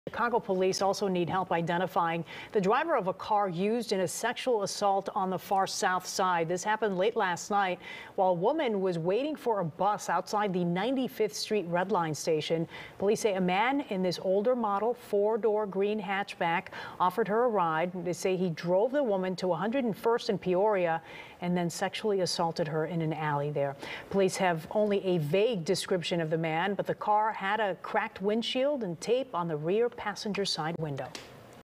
Police: Woman picked up at CTA bus stop sexually assaulted by driver
Chicago police seek help identifying the driver of a car used in a sexual assault on the Far South Side.